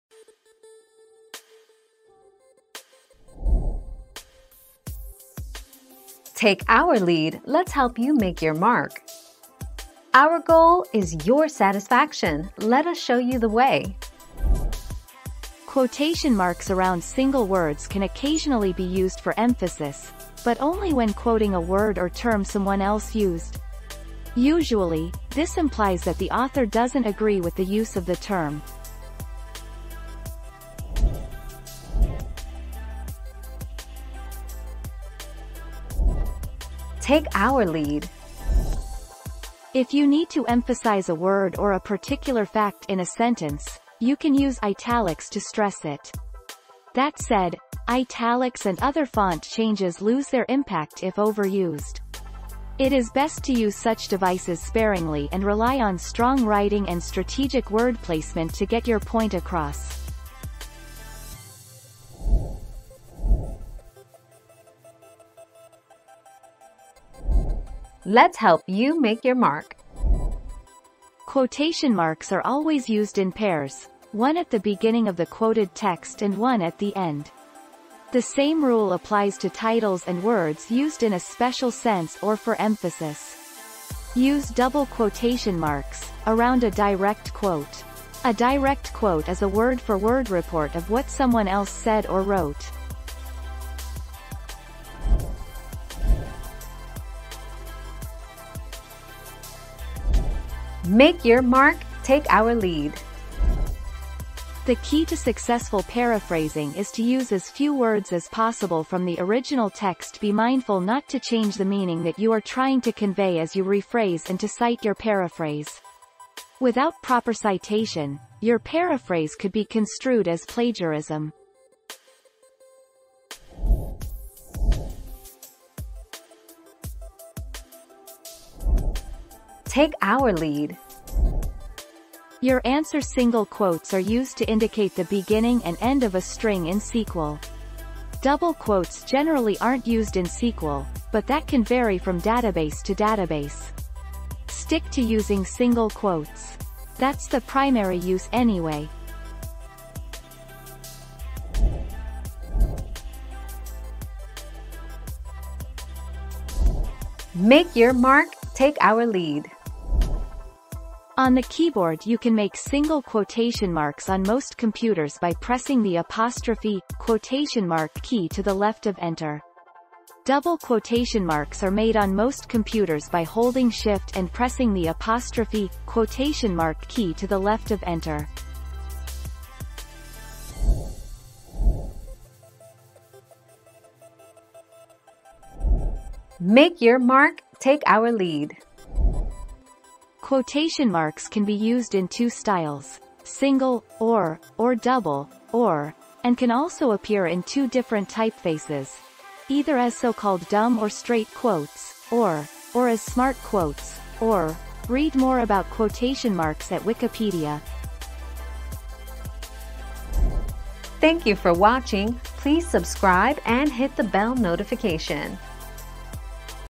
Can you use single quotes for emphasis?
00:00 - Can you use single quotes for emphasis?
00:32 - How do you emphasize a word?
01:04 - How do you use double quotes in a sentence?
01:36 - How do you paraphrase correctly?
02:05 - Which should be in double quotes in SQL?
02:32 - How do you make single quotes on a keyboard?
03:01 - What is a single quote called?

Laura S. Harris (2021, July 6.) Can you use single quotes for emphasis?
    AskAbout.video/articles/Can-you-use-single-quotes-for-emphasis-257045